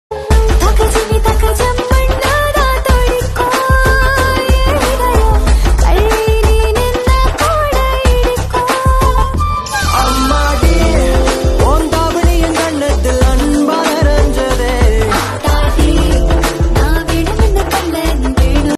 Gown | Ballgown | Diadem
@sivaangi.krish flaunting our DiademGown in her own cute, pretty way! 🥰

Onion pink foil gown enhanced with stones and beads is the best choice for your big day. Be it a party or a wedding, go slay in this stunner! ✨

📌𝐃𝐢𝐚𝐝𝐞𝐦: Nungambakam, Opp. to The Park hotel, Gemini flyover, Chennai - 600 006